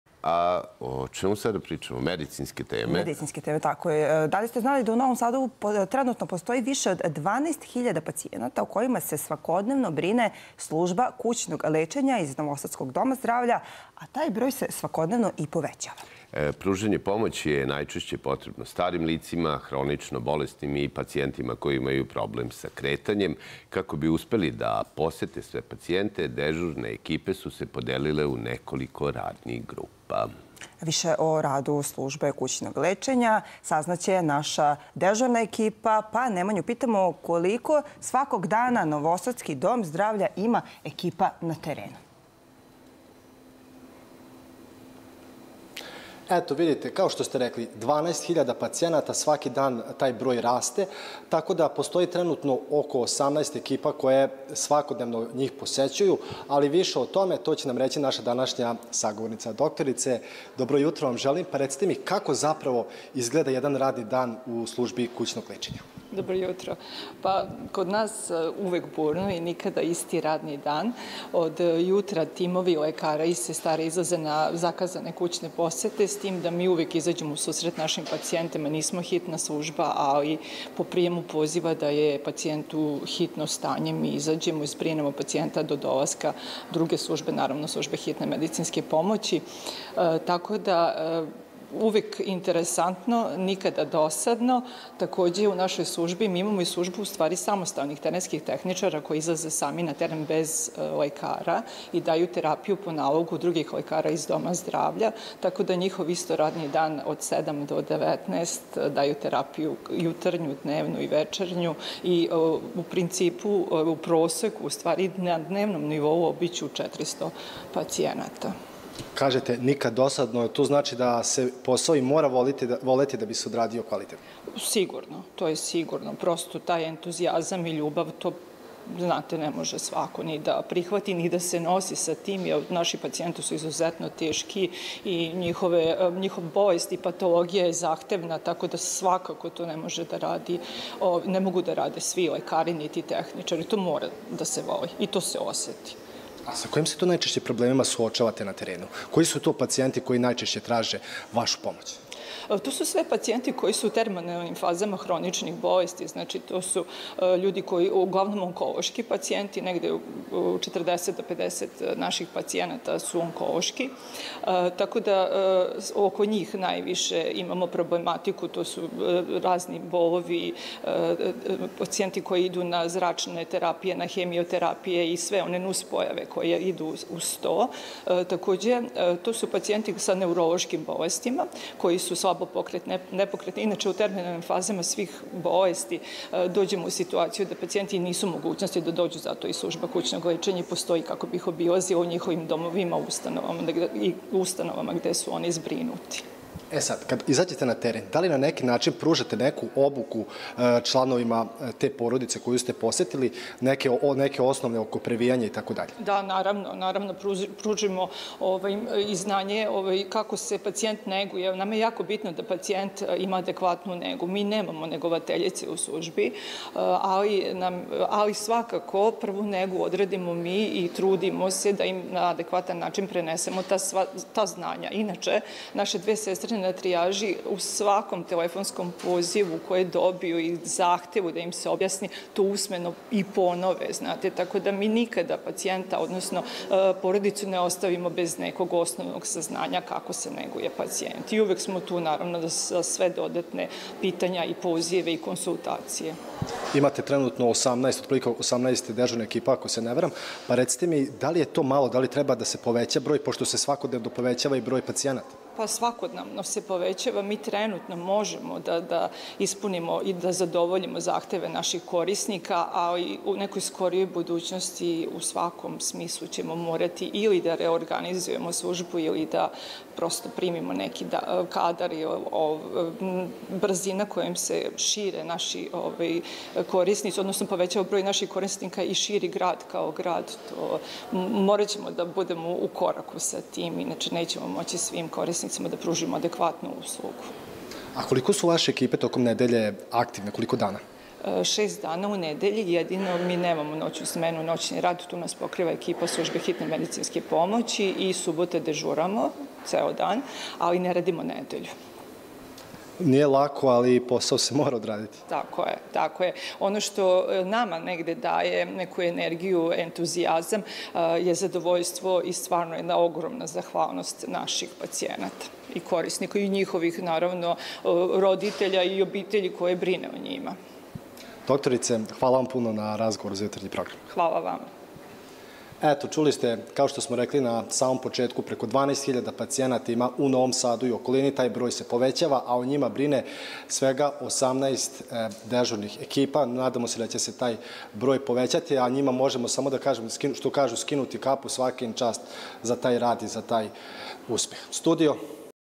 Koliko Novi Sad ima kućnih lekara
U Novom Sadu trenutno postoji više od 12 hiljada pacijenata o kojima se svakodnevno brine služba kućnog lečenja iz novosadskog Doma zdravlja, a taj broj se svakodnevno povećava.  Pružanje pomoći je najčešće potrebno starim licima, hronično bolesnim i pacijentima koji imaju problem sa kretanjem. Kako bi uspeli da posete sve pacijente, dežurne ekipe su se podelile u nekoliko radnih grupa. Koliko svakog dana novosadski Dom zdravlja ima ekipa na terenu?